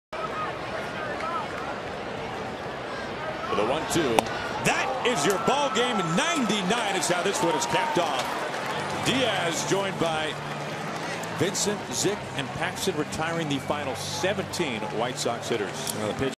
SEA@CWS: Diaz K's Garcia, notches the save
Edwin Diaz strikes out Avisail Garcia in the bottom of the 9th to end the game, securing the Mariners' 4-2 win over the White Sox